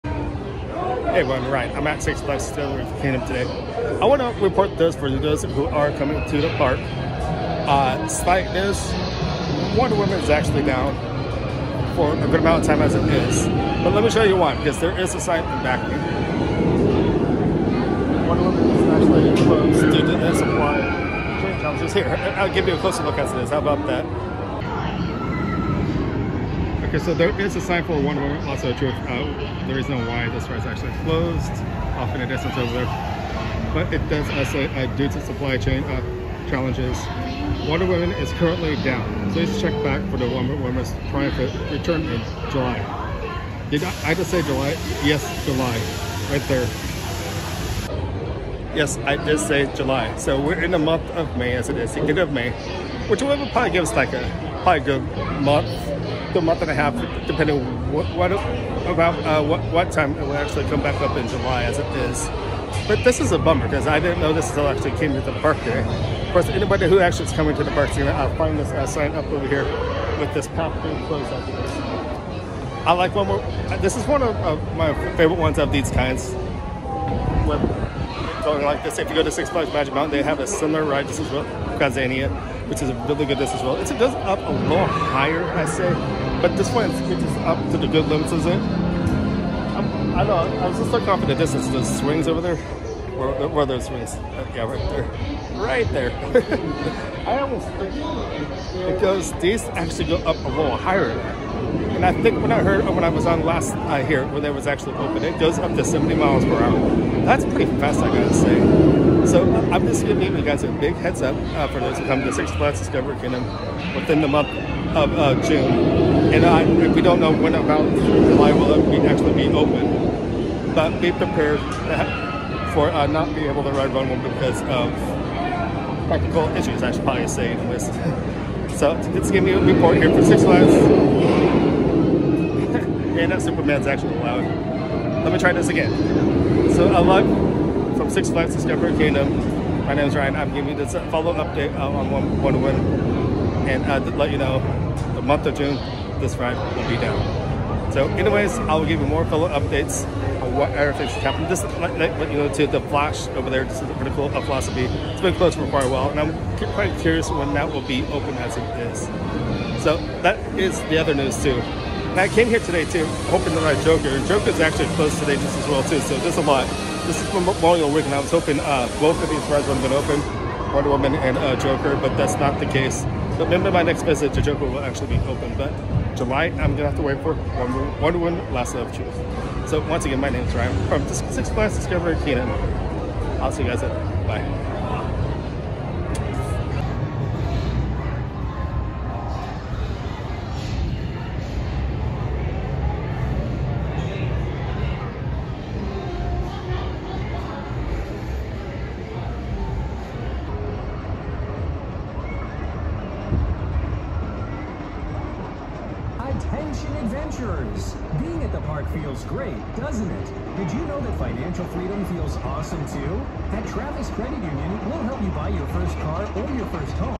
Wonder Woman: Lasso of Truth (Maintenance Closure) at Six Flags Discovery Kingdom
Wonder Woman: Lasso of Truth is closed for about a month or so due to technical issues but it will be back just in time for the summer fun at Six Flags Discovery Kingdom. This is an update at the park on what is going on with Wonder Woman's ride.